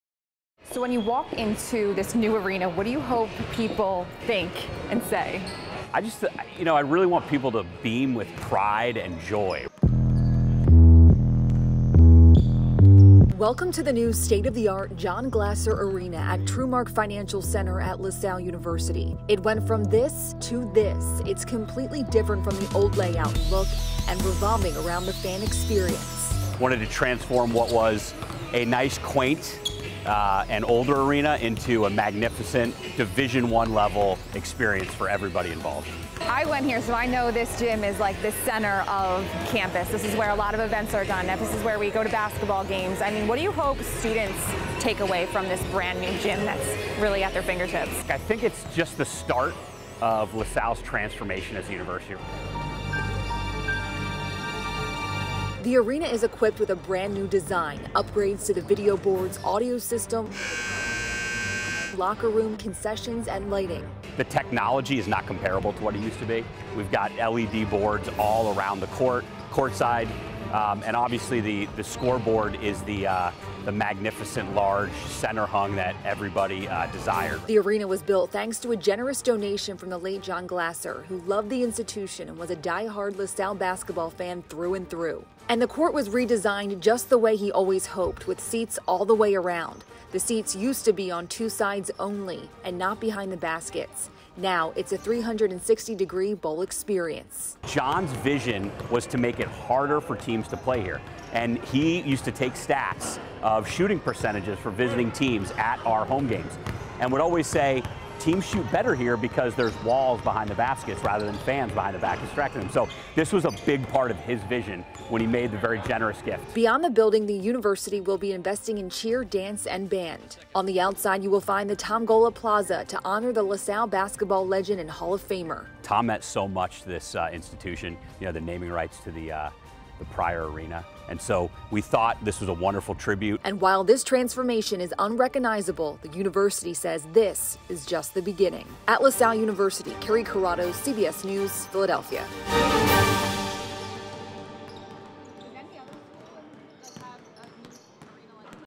La Salle's newly renovated John Glaser Arena puts the fan experience at the forefront
There's a new exploration underway for the La Salle University community. This season, the home of the university's men's and women's basketball teams has a brand-new namesake and state-of-the-art design.